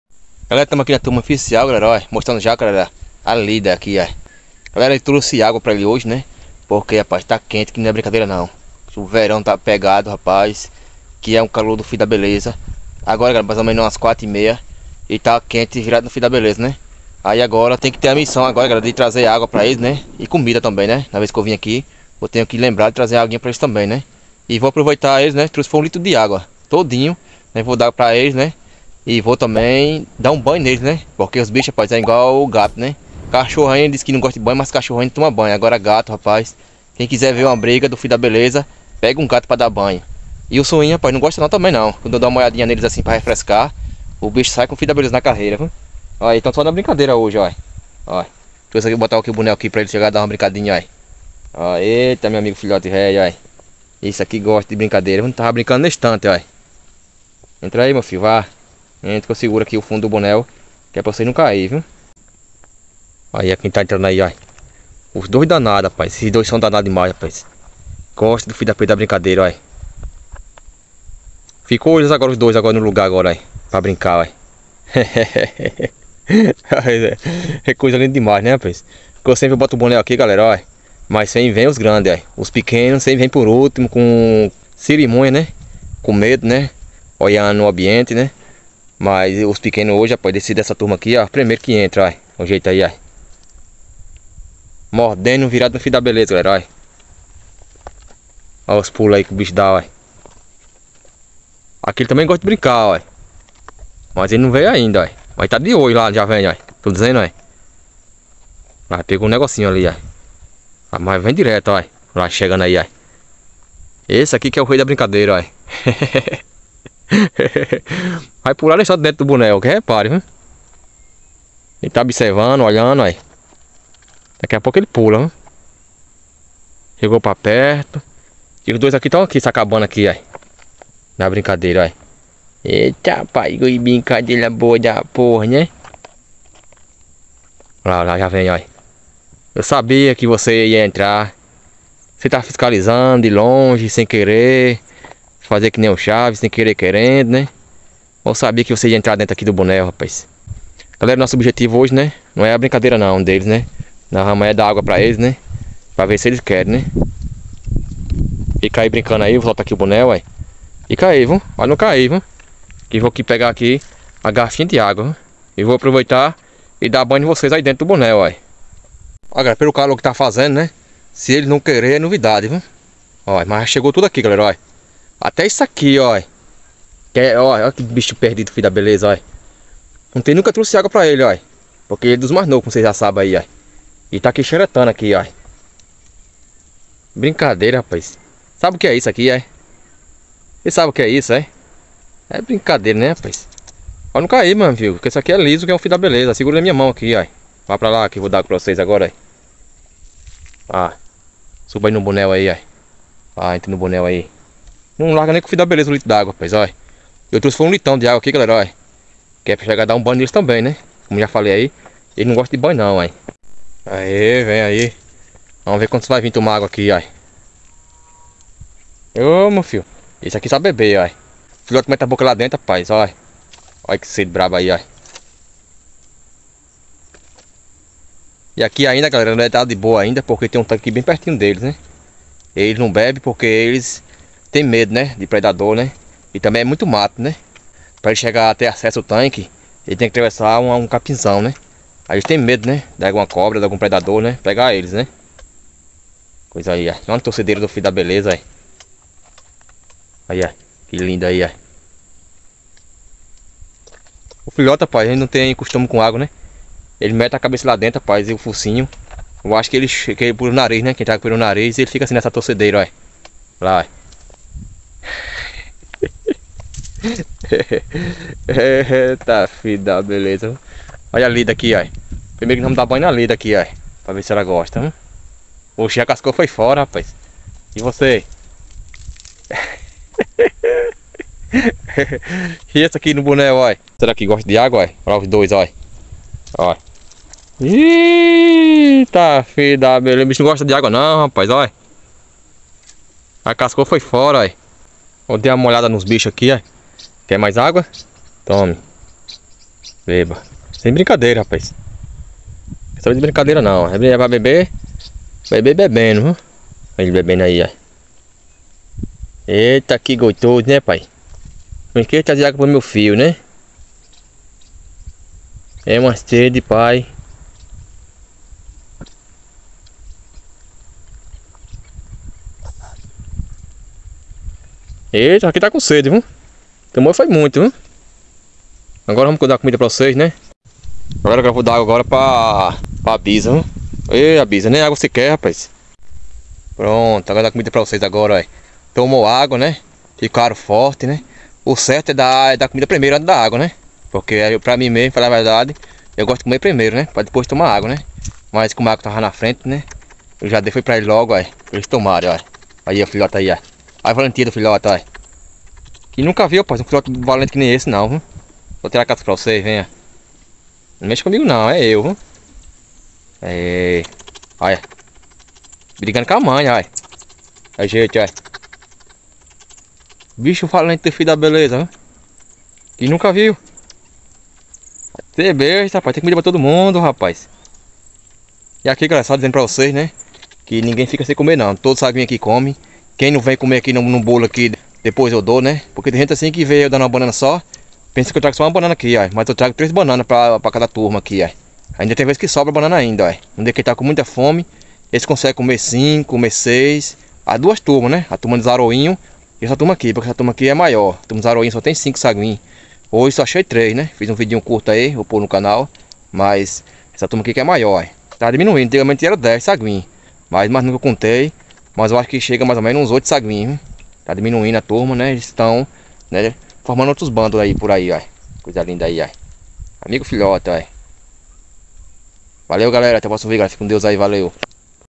Levei água para os sagui, dei até um banho e dois filhotes só quem brinca.
sagui filhotes macaco soim primata saguichamando callithrixjacchus filhote saguideestimação / Veja a sede que os macacos sagui estava, os filhotes de soim com muita sede, e joguei um pouquinho de água para refrescar o pelo dos nico.

Quem quiser ajudar o canal.

Chave pix é.